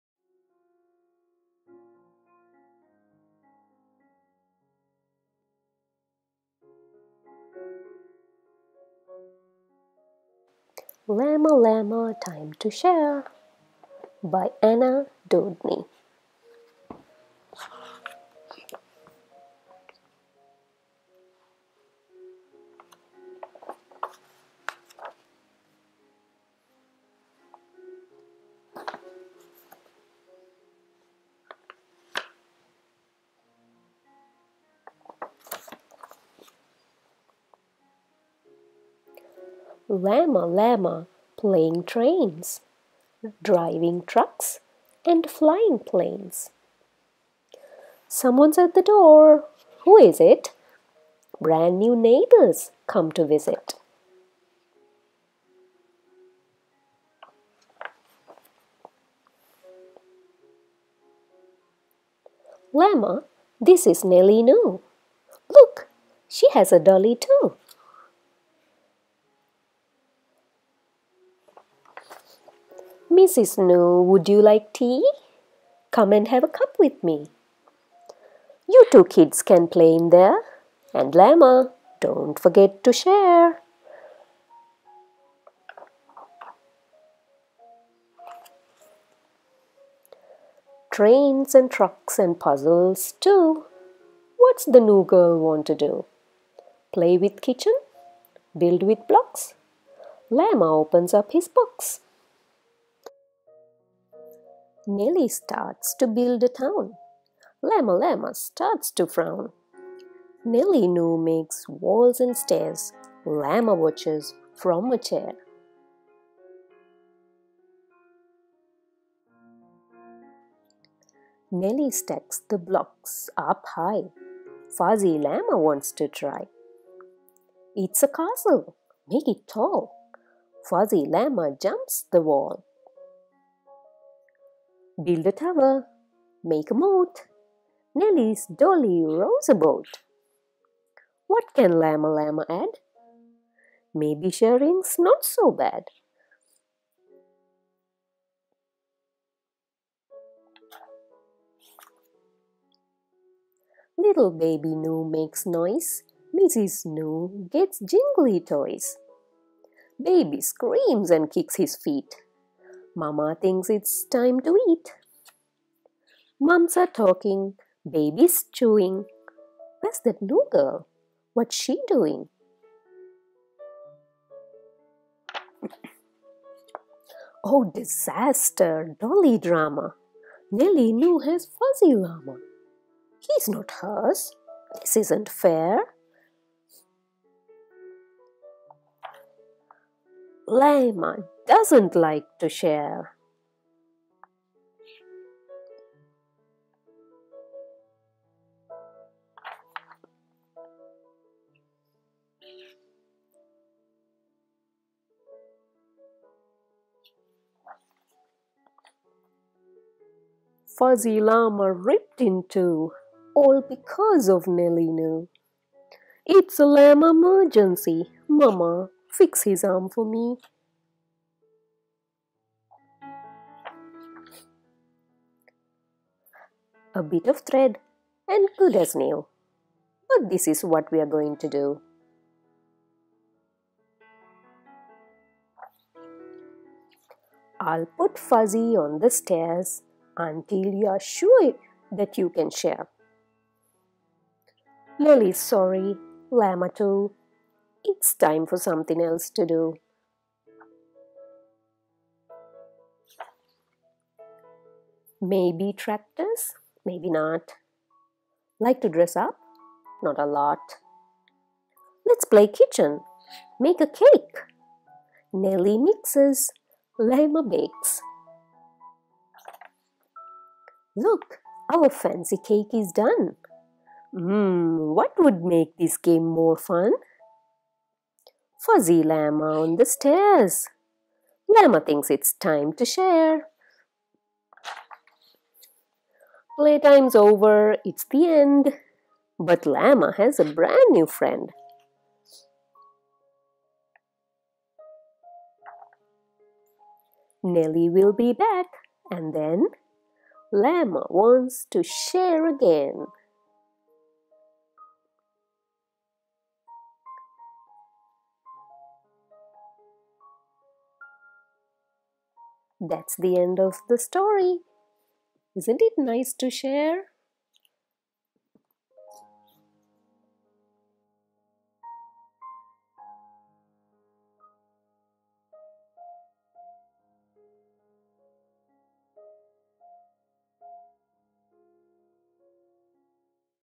Llama llama time to share
This is a beautiful story about how little Llama learns to share with his brand new friend, Nelly.